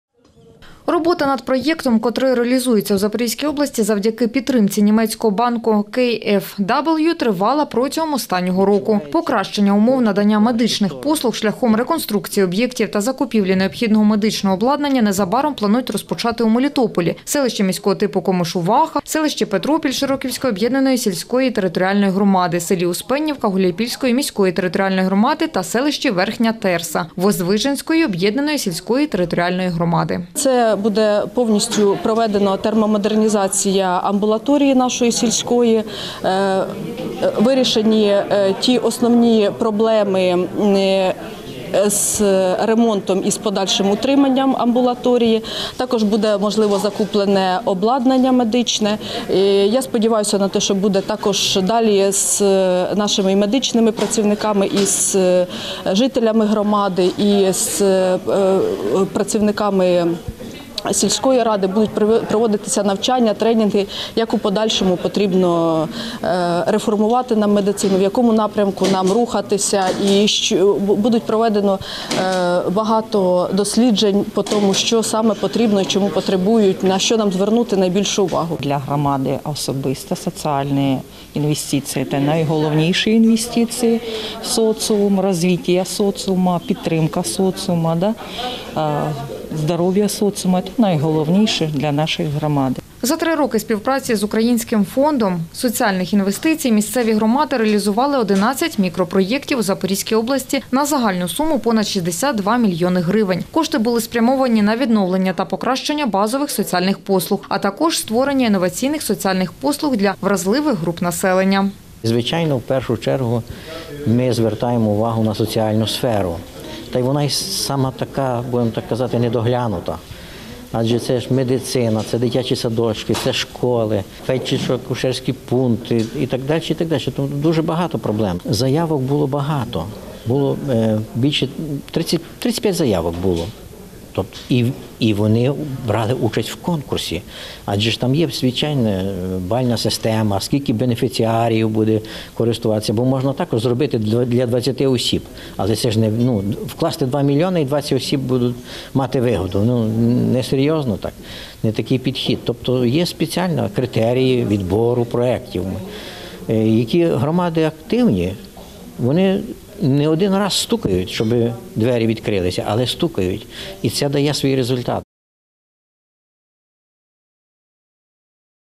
Меморандум про умови надання медичних послуг|Новини|7.10.2020
Робота над проектом, котрий реалізується у Запорізькій області завдяки підтримці німецького банку «KFW», тривала протягом останнього року.